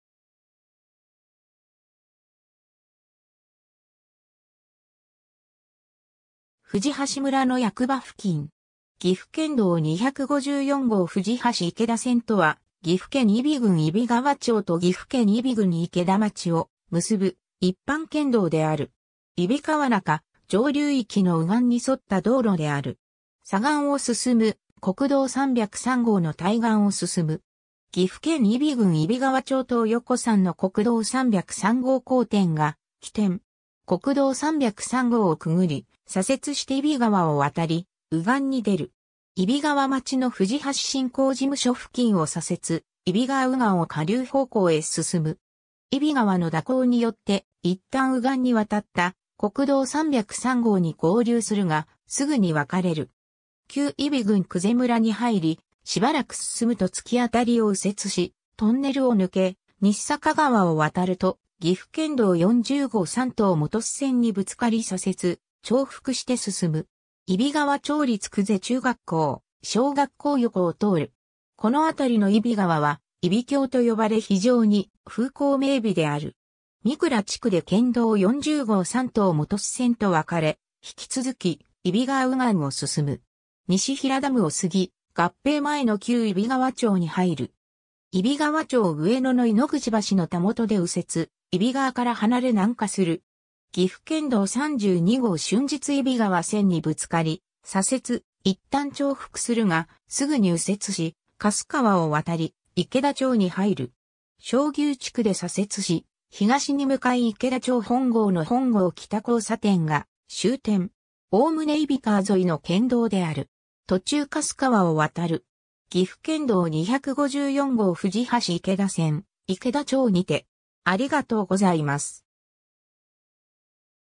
岐阜県道254号藤橋池田線
藤橋村（現･揖斐郡揖斐川町の町域）の役場付近。
 岐阜県道254号藤橋池田線（ぎふけんどう254ごう ふじはしいけだせん）とは、岐阜県揖斐郡揖斐川町と岐阜県揖斐郡池田町を結ぶ一般県道である。
 揖斐川中・上流域の右岸に沿った道路である。
左岸を進む国道303号の対岸を進む。
 岐阜県揖斐郡揖斐川町東横山の国道303号交点が起点。
国道303号をくぐり、左折して揖斐川を渡り、右岸に出る。
揖斐川町の藤橋振興事務所（旧藤橋村役場）付近を左折、揖斐川右岸を下流方向へ進む。
揖斐川の蛇行によって一旦右岸に渡った国道303号に合流するが、すぐに分かれる。
旧揖斐郡久瀬村に入り、しばらく進むと突き当りを右折し、トンネルを抜け、日坂川を渡ると岐阜県道40号山東本巣線にぶつかり左折、重複して進む。
揖斐川町立久瀬中学校・小学校横を通る。
このあたりの揖斐川は揖斐峡と呼ばれ非常に風光明媚である。
三倉地区で県道40号山東本巣線と分かれ、引き続き揖斐川右岸を進む。
西平ダムを過ぎ、合併前の旧揖斐川町に入る。
揖斐川町上野の井ノ口橋のたもとで右折、揖斐川から離れ南下する。
岐阜県道32号春日揖斐川線にぶつかり左折、一旦重複するが、すぐに右折し、粕川（揖斐川支流）を渡り、池田町に入る。
小牛地区で左折し、東に向かい池田町本郷の本郷北交差点（国道417号交点）が終点。
 概ね揖斐川沿いの県道である。
途中粕川を渡る。
 岐阜県道254号藤橋池田線、池田町にて